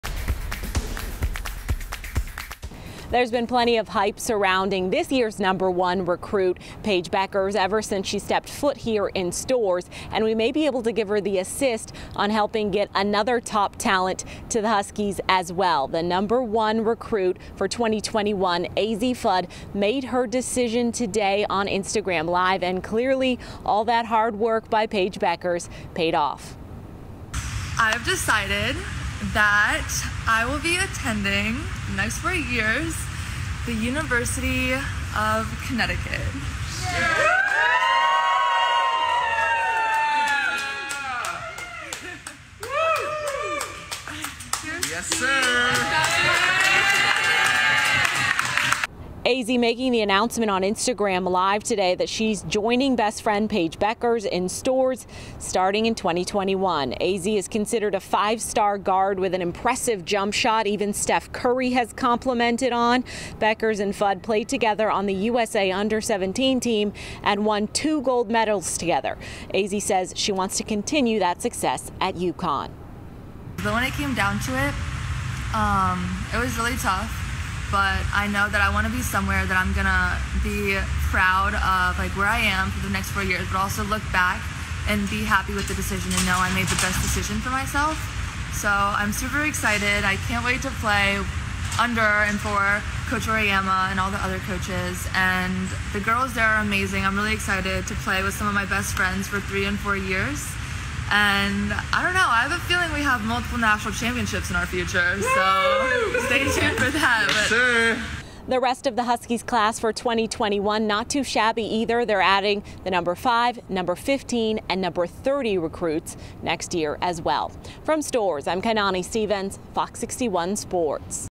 No. 1 recruit, DC native Azzi Fudd picks UConn to play college basketball
St. John's High School Basketball Star Picks the University of Connecticut to Play College Basketball